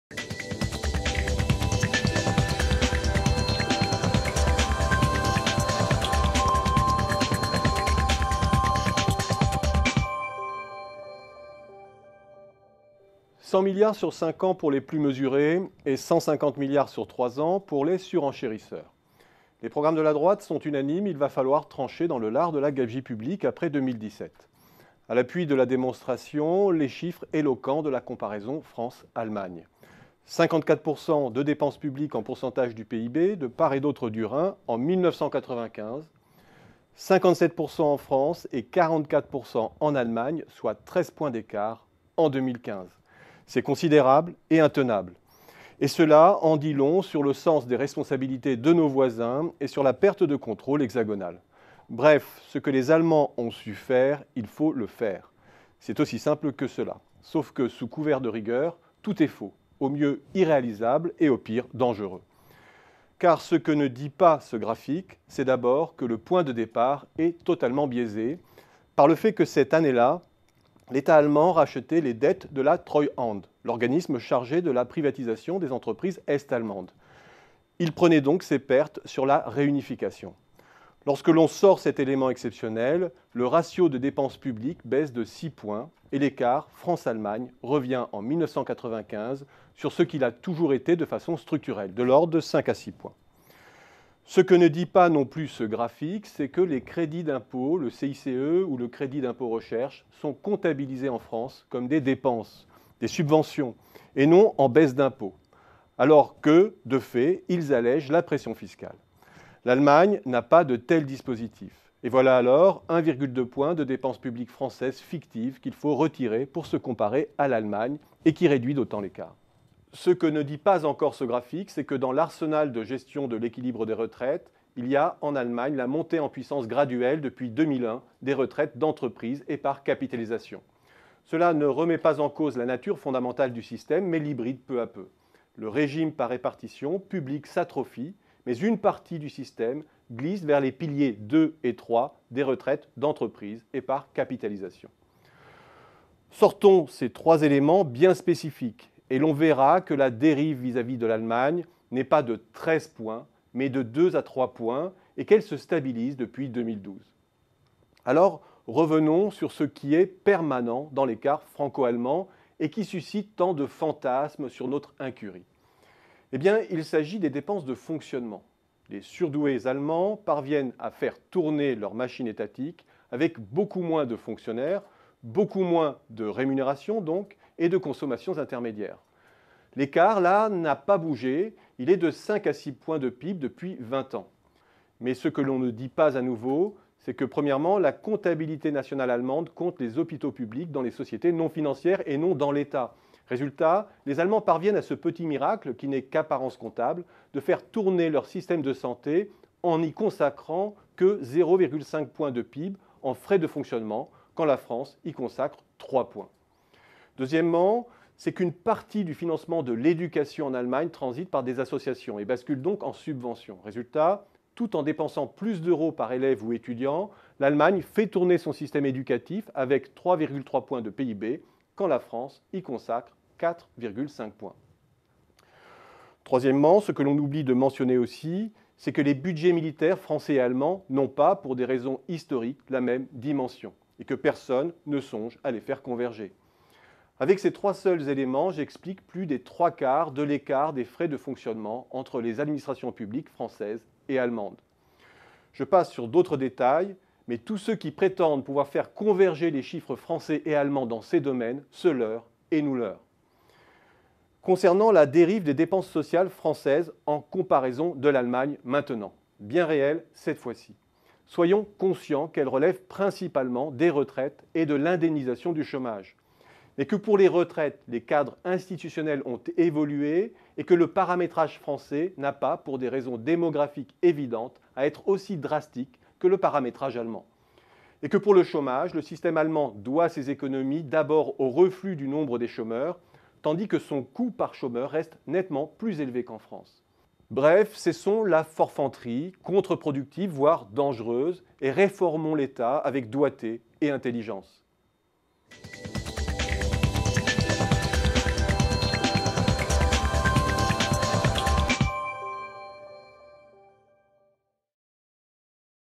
Olivier Passet, Incurie budgétaire française vertu allemande les statistiques tronquées
100 milliards sur 5 ans pour les plus mesurés… 150 milliards sur 3 ans pour les surenchérisseurs. Les programmes de la droite sont unanimes. Il va falloir trancher dans le lard de la gabegie publique après 2017. A l’appui de la démonstration les chiffres éloquents de la comparaison France Allemagne. : 54 % de dépenses publiques en % du PIB de part et d’autre du Rhin en 1995. 57% en France et 44% en Allemagne : soit 13 points d’écart en 2015. C’est considérable et intenable. Et cela en dit long sur le sens des responsabilités de nos voisins et sur la perte de contrôle hexagonal. Bref, ce que les Allemands ont su le faire… il faut le faire. C’est aussi simple que cela. Sauf que sous couvert de rigueur, tout est faux, au mieux irréalisable et, au pire dangereux. [...]